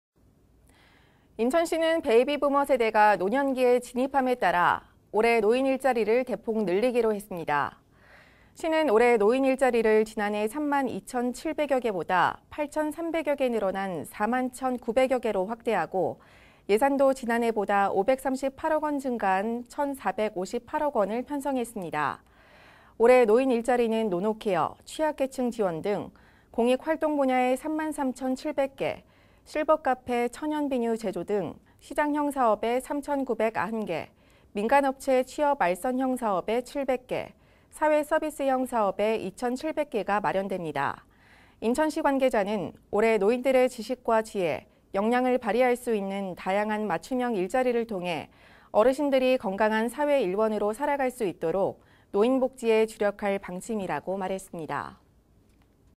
[NIB뉴스] 인천시, 올해 노인일자리 4만1천90개로 확대... 8천371개 증가
인천시가 베이비부머 세대가 노년기에 진입함에 따라 올해 노인일자리를 대폭 늘리기로 했습니다.

시는 올해 노인일자리를 지난해 3만2천719개보다 8천371개 늘어난
4만1천90개로 확대하고, 예산도 지난해 보다 538억 원 증가한
1천458억 원을 편성했습니다.

올해 노인일자리는 노노케어, 취약계층 지원 등 공익활동 분야에
3만3천700개, 실버카페·천연비누 제조 등 시장형 사업에 3천990개, 민간업체 취업알선형 사업에 700개, 사회서비스형 사업에 2천700개가 마련됩니다.

인천시 관계자는 “올해 노인들의 지식과 지혜, 역량을 발휘할 수
있는 다양한 맞춤형 일자리를 통해, 어르신들이 건강한 사회 일원으로 살아갈 수 있도록 노인 복지에 주력할 방침”이라고 말했습니다.